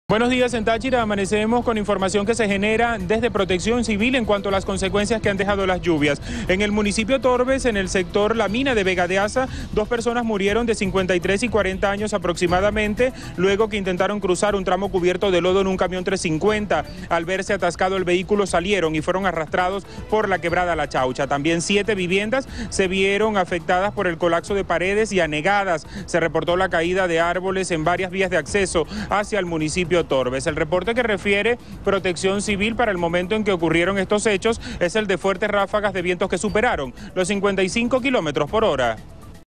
En Táchira 2 personas murieron como consecuencia de las lluvias
En Táchira en el municipio Tórbes en el sector La Mina en Vega de Aza, dos personas murieron, de 53 y 40 años aproximadamente, luego que intentaran cruzar un tramo cubierto de lodo en un camión 350. Al verse atascado el vehículo salieron y fueron arrastrados por la quebrada La Chaucha.